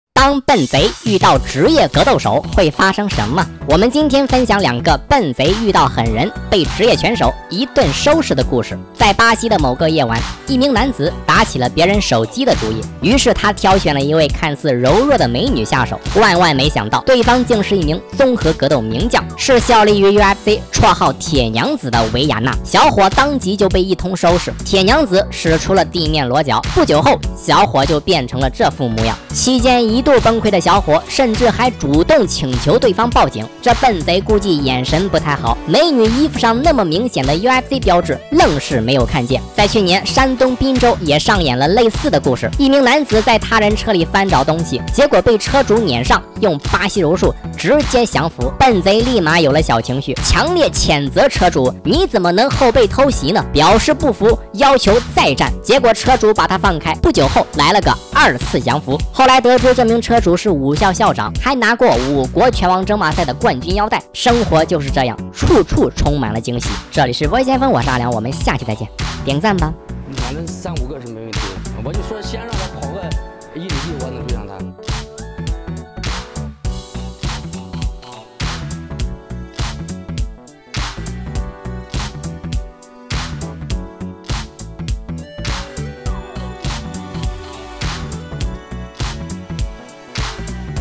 蠢贼要抢美女的手机，殊不知对方是UFC选手，被按在地上胖揍 【搏击先锋】
当笨贼遇到职业格斗手会发生什么？
我们今天分享两个 笨贼遇到狠人
被职业拳手一顿收拾的故事
在巴西的某个夜晚
一名男子打起了别人手机的主意
于是他挑选了一位看似柔弱的美女下手
万万没想到对方竟是一名综合格斗名将
是效力于UFC 绰号“铁娘子”的韦雅娜
小伙当即就被一通收拾
铁娘子使出了地面裸绞
不久后小伙就变成了这副模样
期间一度崩溃的小伙甚至还主动请求对方BJ
这笨贼估计眼神不太好
美女衣服上这么明显的UFC标志愣是没有看见
在去年山东滨州也上演了类似的故事
一名黑衣男子在他人车里翻找东西
结果被车主撵上用巴西柔术直接降服
笨贼立马有了小情绪
强烈谴责车主 你怎么能后背偷袭呢？
表示不服 要求再战！
结果车主把他放开 不久后来了个二次降服
后来得知这名车主是武校校长
还拿到过五国拳王争霸赛的冠军腰带
生活就是这样 处处充满了惊喜